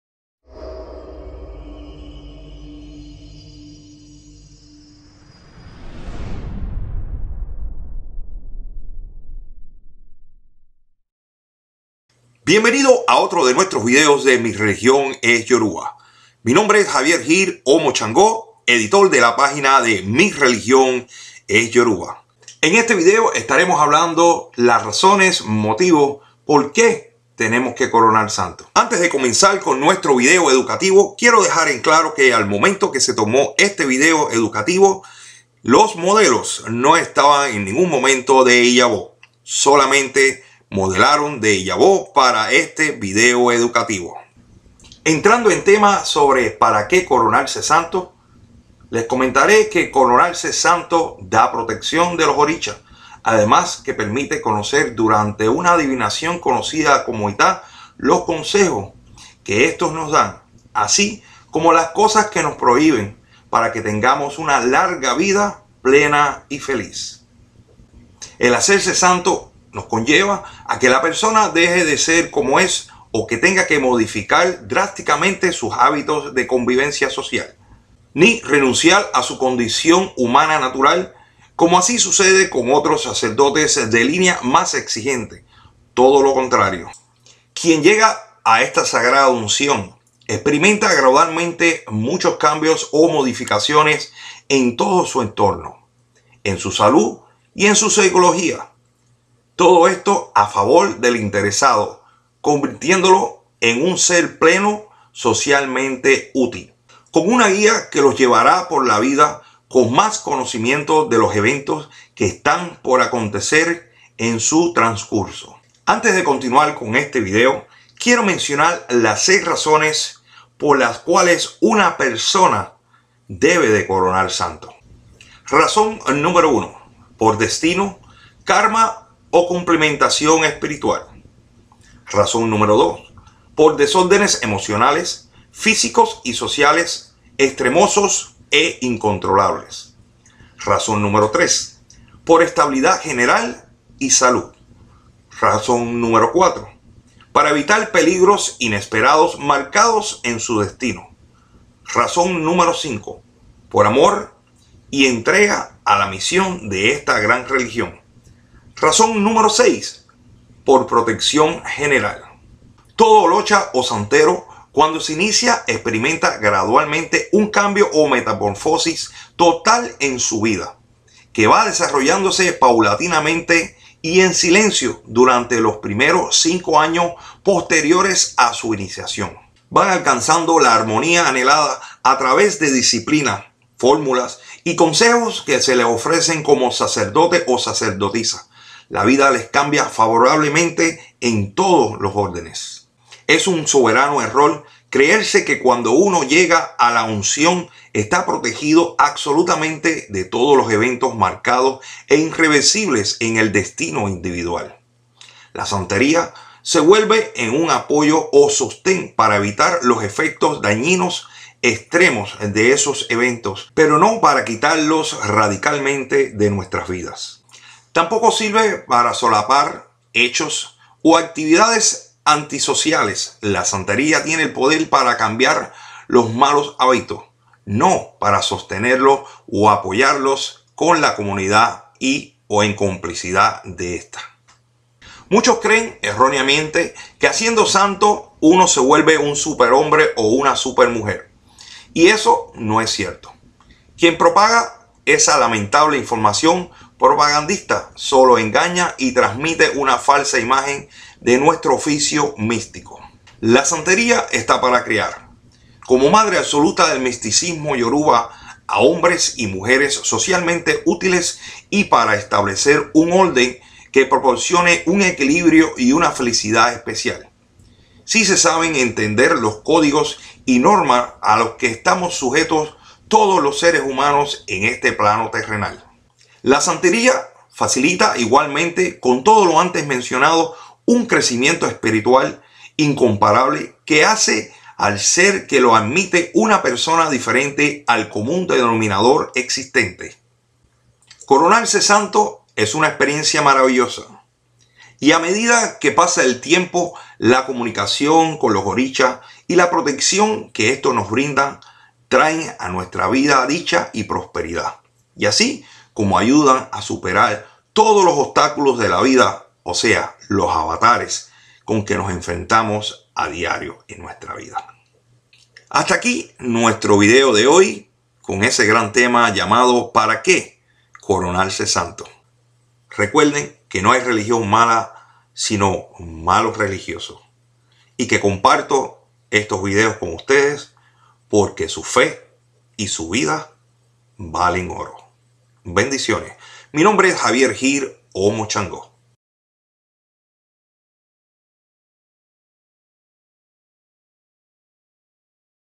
Para que Coronar Santo?
Video Educativo de Nuestra Religion.

NOTA: AL MOMENTO DE TOMAR ESTE VIDEO LOS MODELOS NO ESTAN DE IYAWO SOLO SON MODELOS... VIDEO TOMADO Y EDITADO POR JAVIER GIL OMO SHANGO...

Bendiciones
Javier Gil Omo Shango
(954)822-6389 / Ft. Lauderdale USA
Liz Garcia Omo Yemaya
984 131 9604 / Mérida Yucatán México